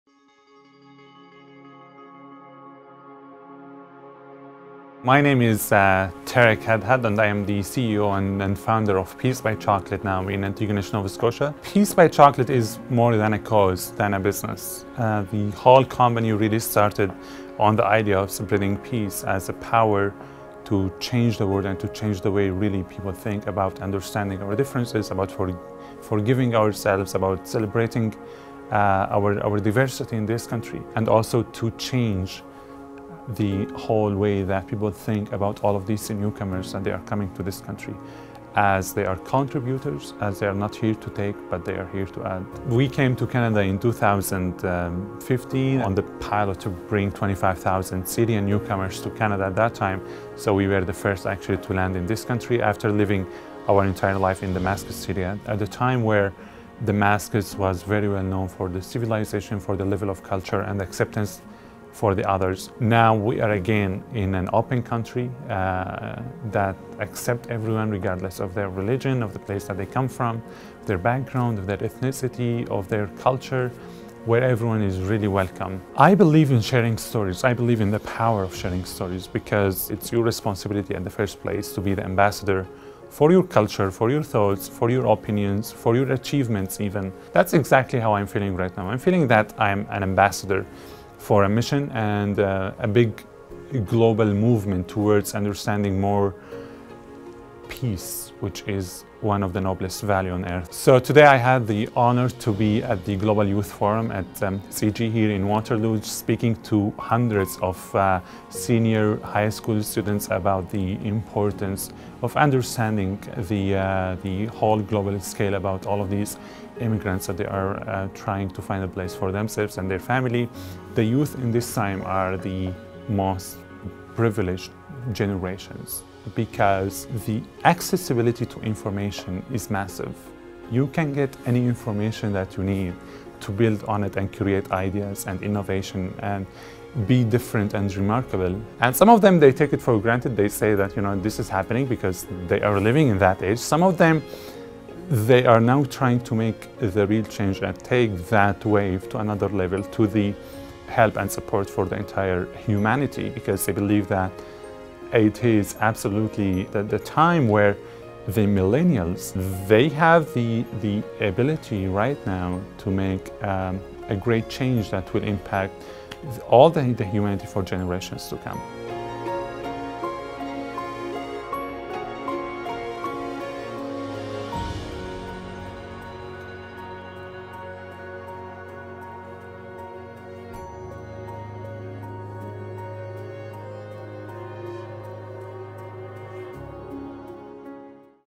Tareq Hadhad at the Global Youth Forum
Hosted by CIGI and the Balsillie School of International Affairs, the Global Youth Forum is a one-day conference for senior high school students. This year’s forum took place on December 14th, 2018 and focused on refugees in a global context.

Tareq Hadhad, founder and CEO of Peace by Chocolate was the keynote speaker at the forum. For more than 20 years the Hadhad family shipped specialty treats from Syria to countries all over the Middle East, including Yemen, Jordan and Lebanon. Like much of Syria, the Hadhad chocolate factory was destroyed in a bombing that forced the family to leave everything behind.

The family spent three years at a refugee camp, then settled in Antigonish, Nova Scotia in early 2016, as Canada accepted a wave of more than 25,000 Syrians.

In this video, Hadhad tells the story of how he rebuilt his family’s business in their new home and why he feels it’s important to share his story across the globe and with youth in particular. According to Hadhad, today’s youth — given their access to information — have the ability to make changes that will impact the world for generations to come.